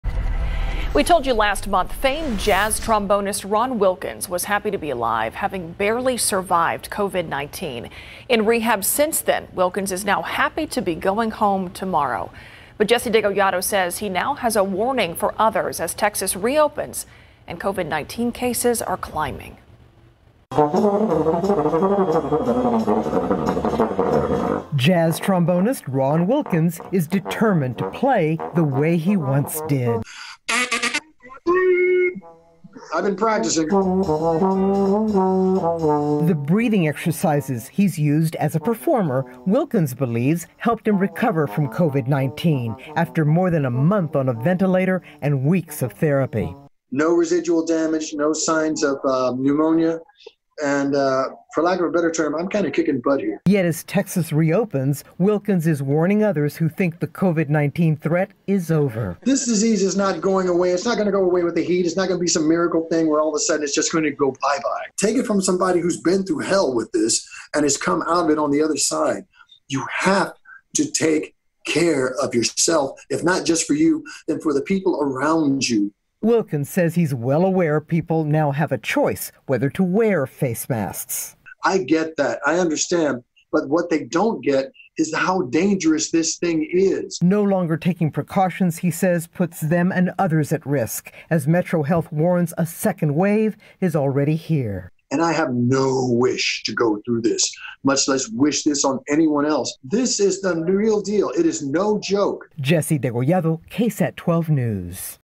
COVID-19 survivor has warning as cases climb in San Antonio, statewide
The world COVID-19 survivor Ron Wilkins will rejoin Tuesday after two months of being hospitalized and undergoing physical and occupational therapy concerns the famed jazz trombonist.